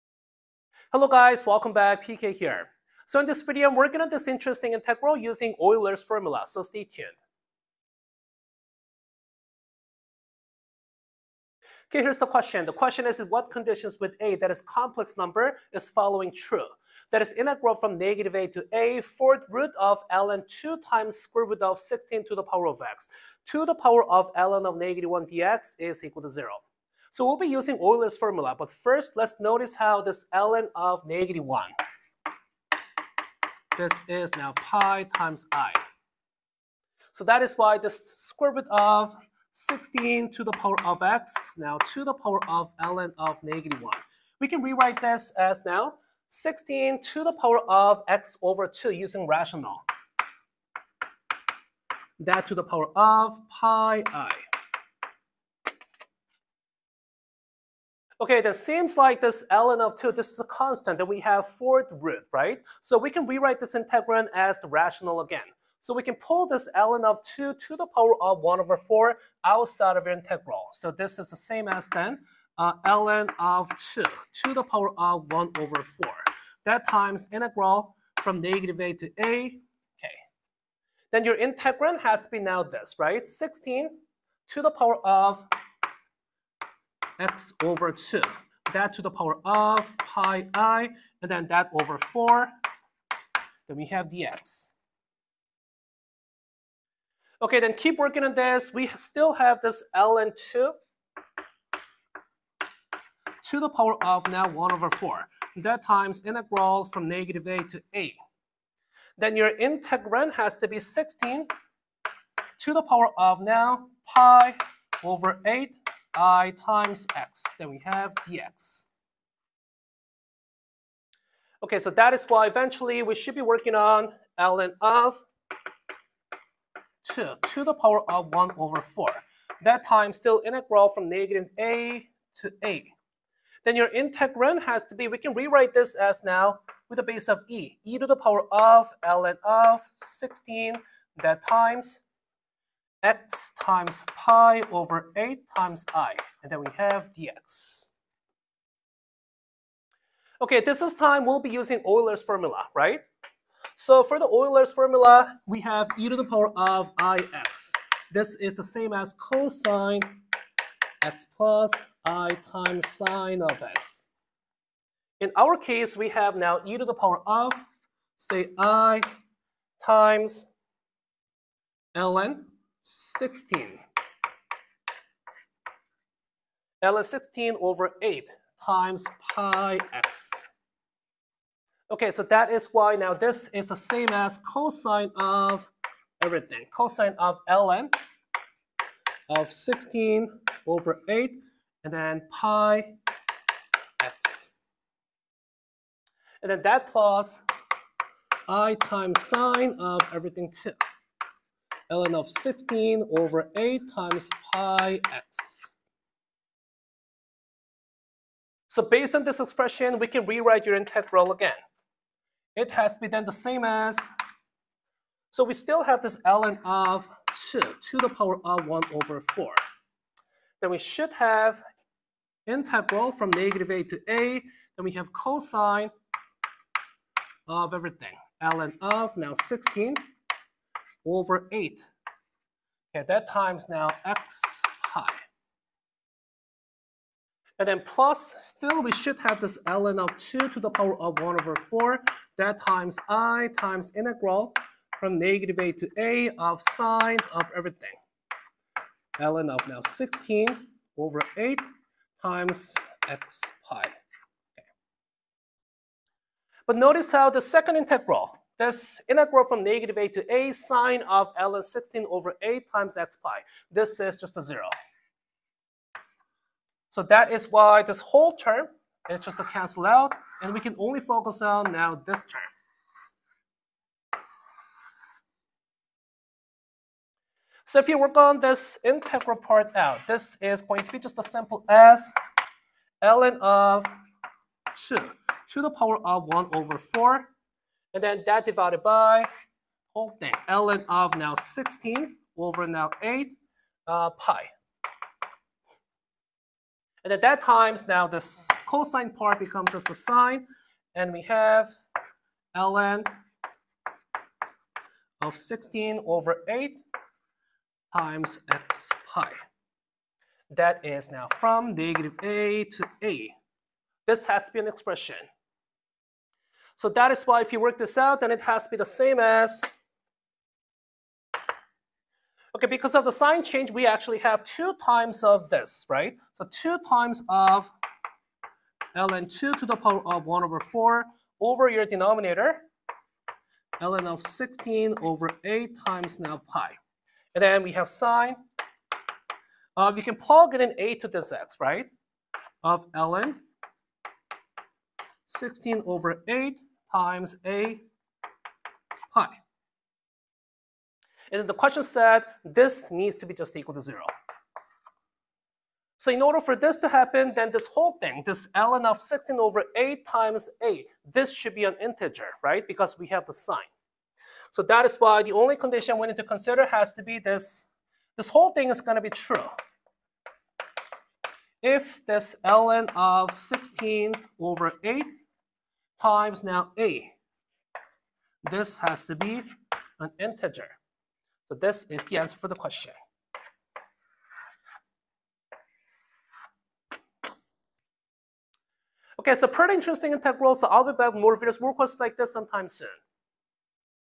Euler's Formula is an Advanced Integration Weapon
In this video, I am evaluating an interesting but strange integral using Euler's formula.